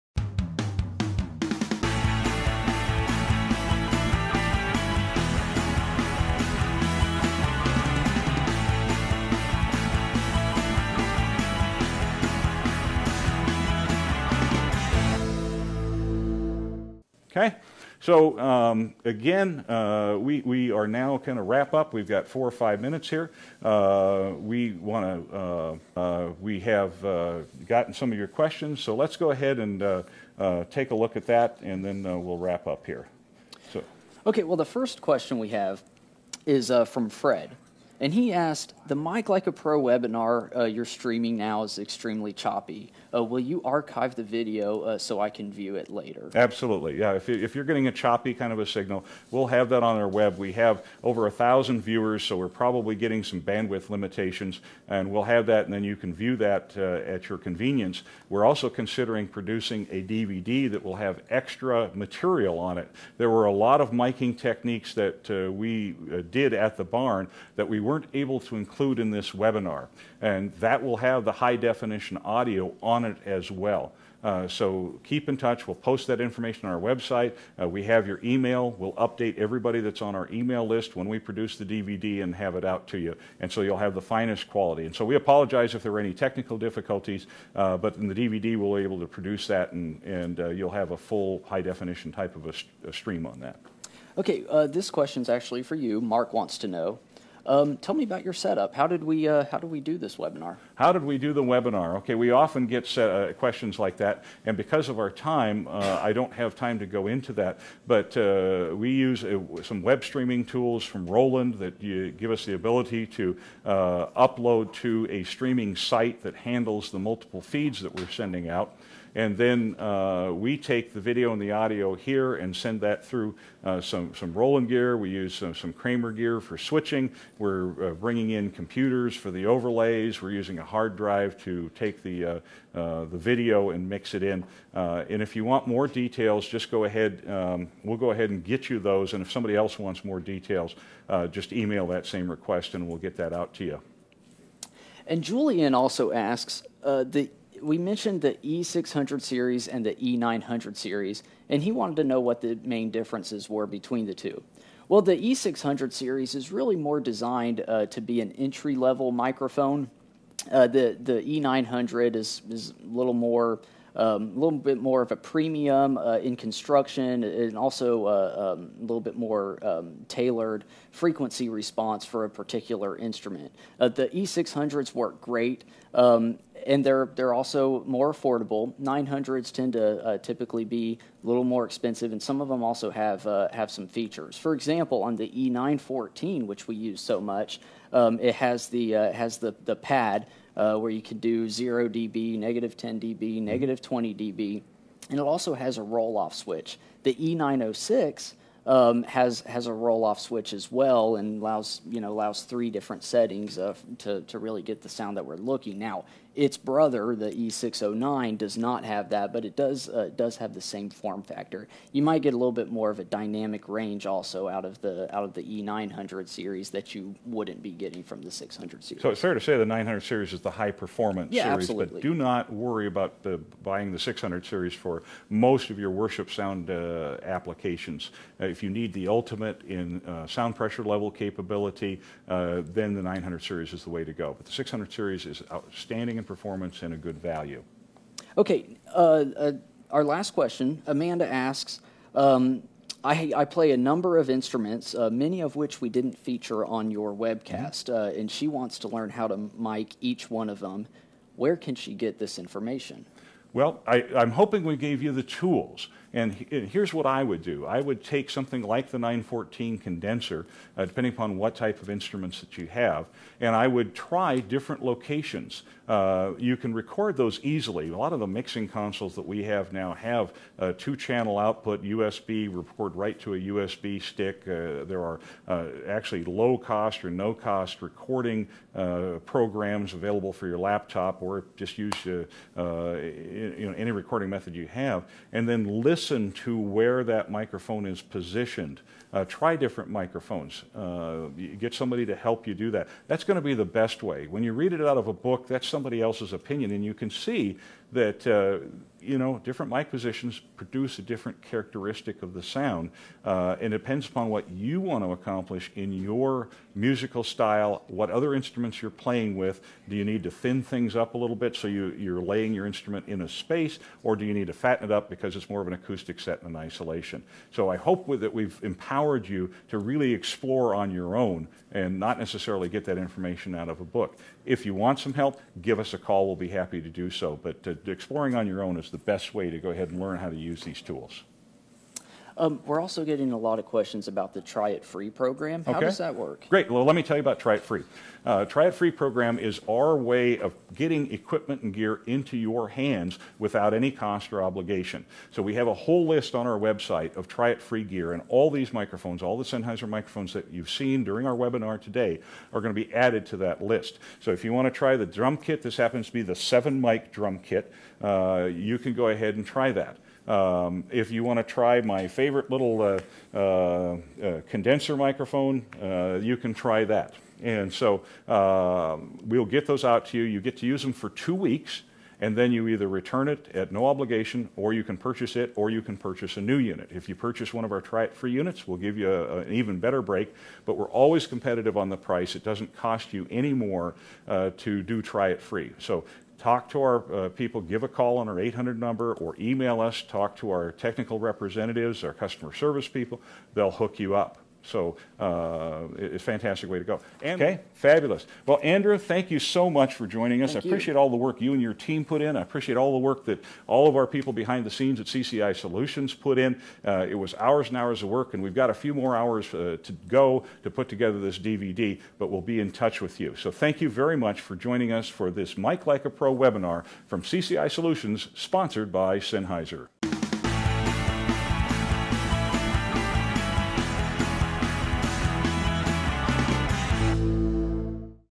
Learn to Mic Like a Pro Part 7 (Wrap Up)
In part seven, we wrap up the Webinar with a review of basic principles presented throughout. Ron & Andrew then answer some of the questions sent in from Webinar viewers. Ron describes our "Try It Free" program and how you can get hands-on experience with various pro A/V gear at your church.